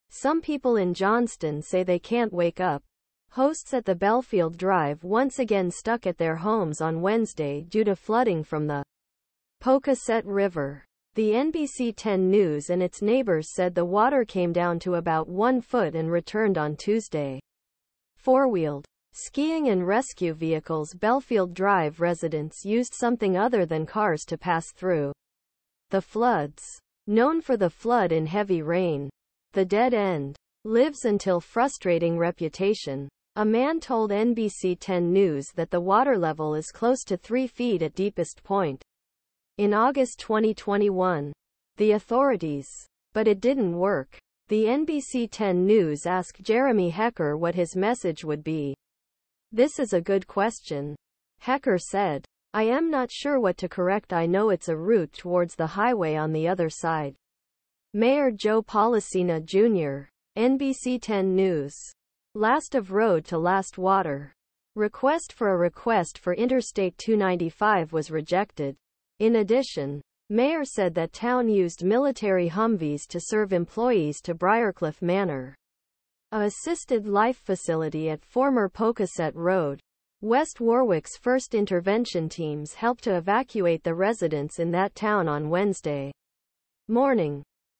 Johnston rescues residents from flooded apartment complex, neighborhood
Some people in Johnston say they can't wake up. Hosts at the Belfield Drive once again stuck at their homes on Wednesday due to flooding from the Pocasset River. The NBC 10 News and its neighbors said the water came down to about 1 feet and returned on Tuesday.  Four-wheeled, skiing and rescue vehicles-Belfield Drive residents used something other than cars to pass through the floods. Known for the flood in heavy rain, the dead end, lives until frustrating reputation. A man told NBC 10 News that the water level is close to 3 feet at deepest point. In August 2021, the authorities, but it didn't work. The NBC 10 News asked Jeremy Hecker what his message would be. "This is a good question," hecker said, "I'm not sure what to correct - I know it's a route towards the highway on the other side. Mayor Joe Polisena Jr., NBC 10 News, last of road to last water, request for a request for Interstate 295 was rejected. In addition, Mayor said that town used military Humvees to serve employees to Briarcliffe Manor, a assisted life facility at former Pocasset Road. West Warwick's first intervention teams helped to evacuate the residents in that town on Wednesday morning.